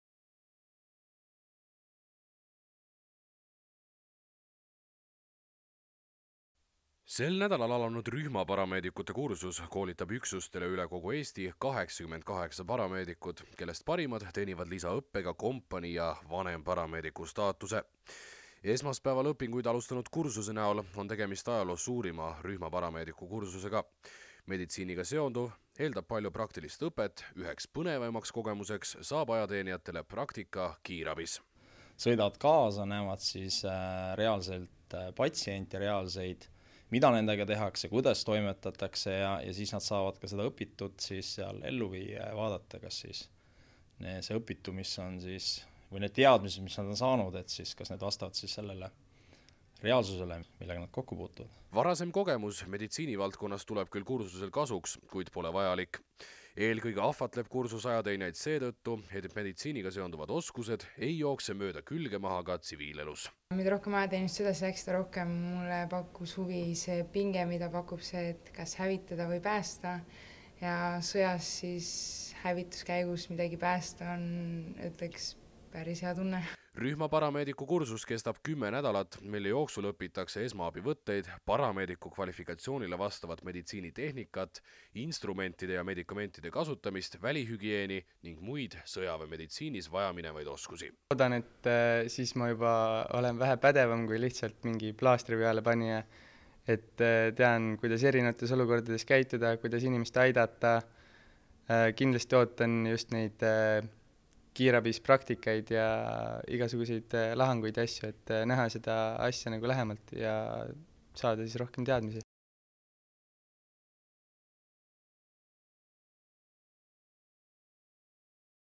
Ajateenijatele algas rühmaparameediku kursus
Sel nädalal algas Ämaris rühmaparameediku kursus, mille käigus koolitatakse üksustele üle Eesti välja 88 rühmaparameedikut. Tegemist on meditsiini väljaõppekeskuse ajaloo suurima rühmaparameediku kursusega.